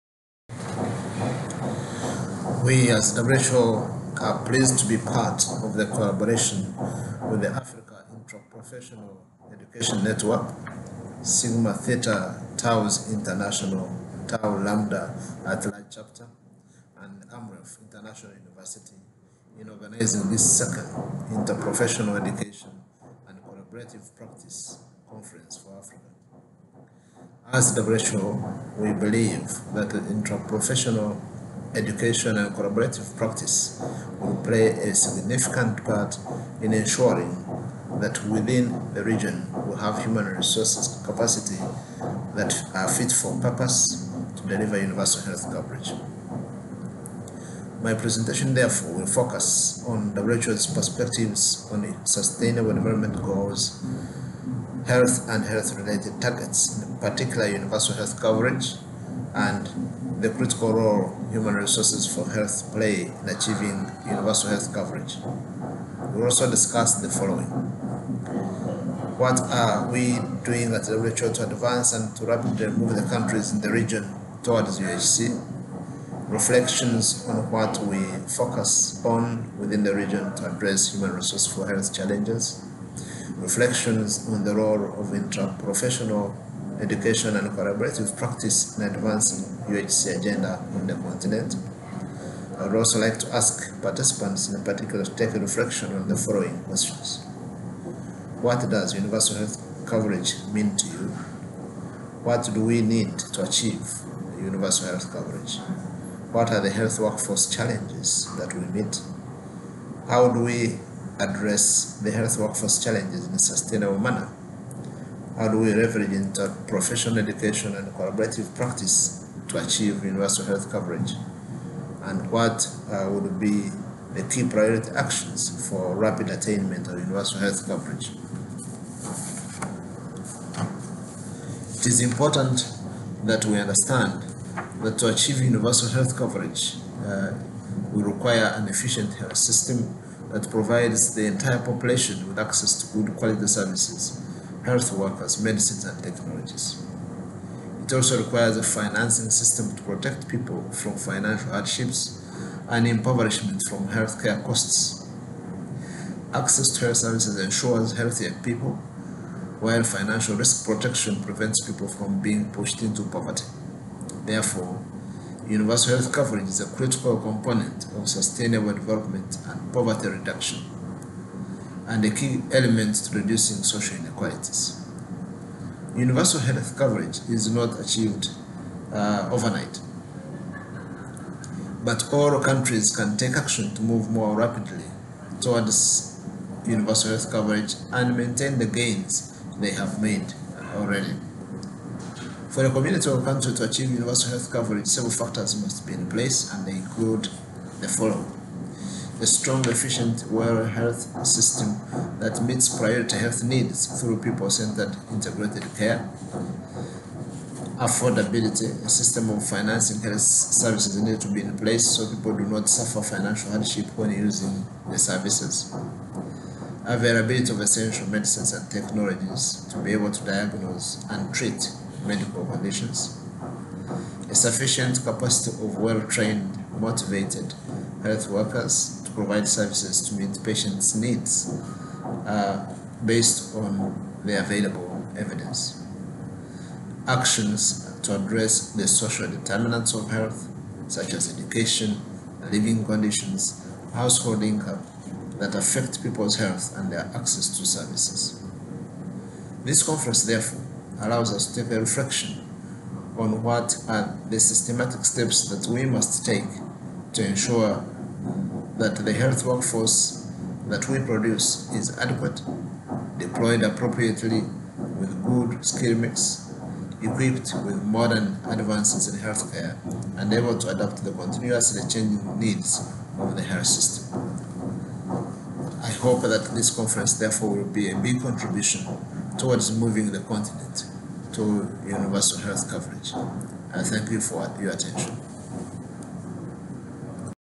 IPECP - Message from Dr Prosper Tumusiime (WHO AFRO)
Promo of the address of Dr Prosper Tumusiime (WHO AFRO) at the Second Interprofessional Education and Collaborative Practice for Africa conference.